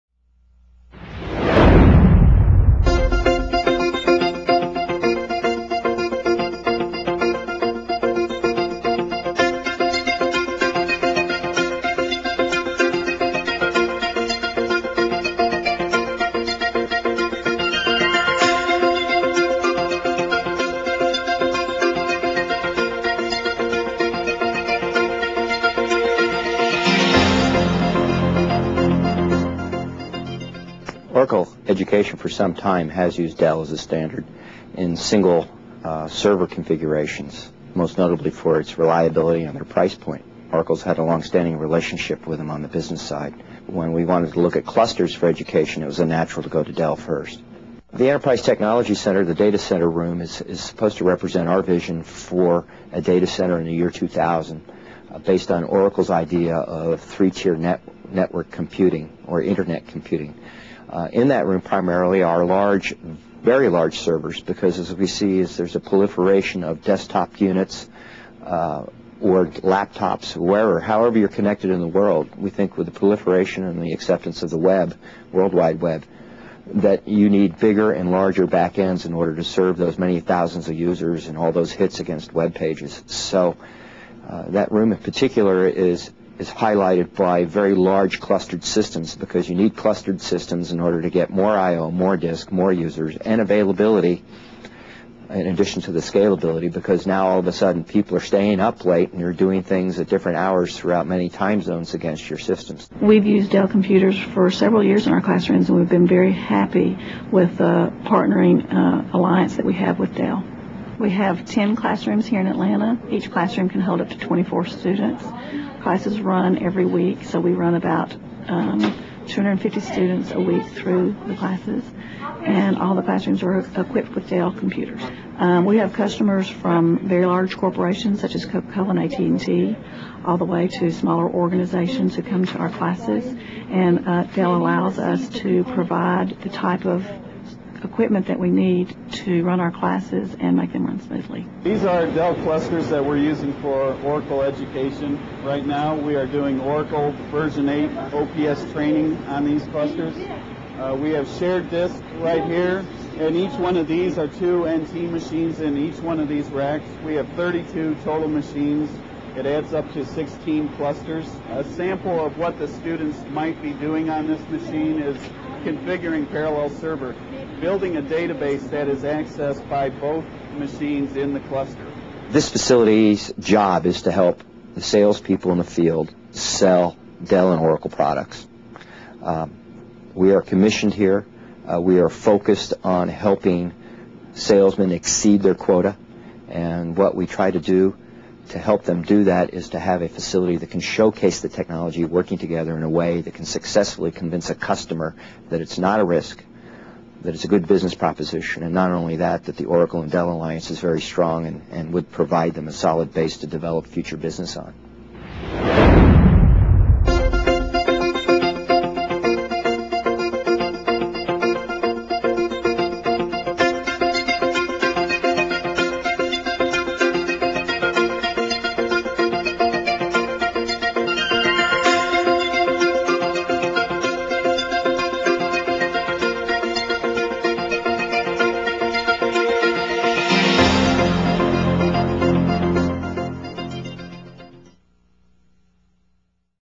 Dell ETC
Scott Hunter, Senior Director at Oracle, explains why Dell was chosen to be a partner in Oracle's Enterprise Technology Center.